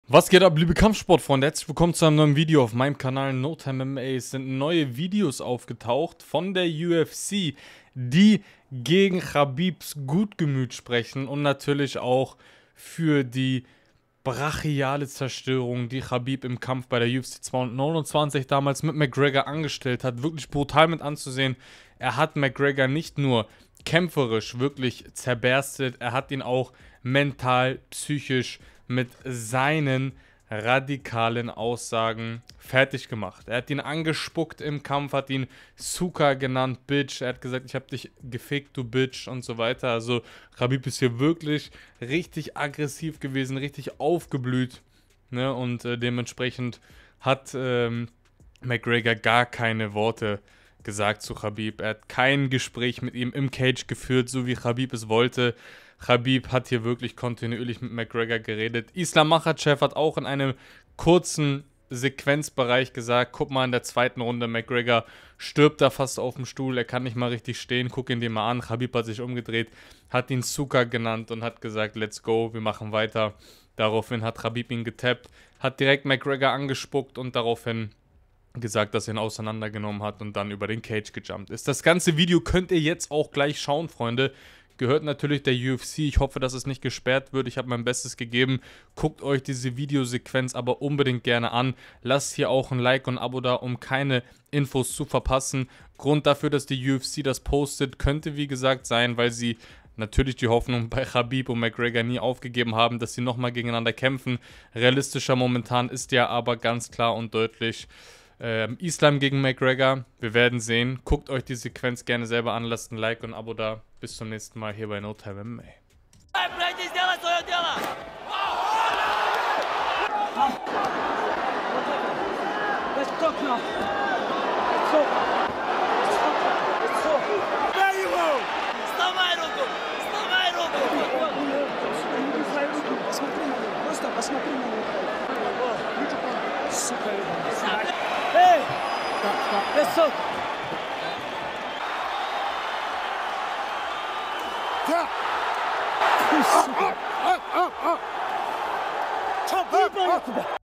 "ICH HABE DICH GEFI*** DU SCHLAM**!" KHABIB SPUCKT CONOR MCGREGOR AN UND BELEIDIGT IHN IM KAMPF!
Euer NoTimeMMA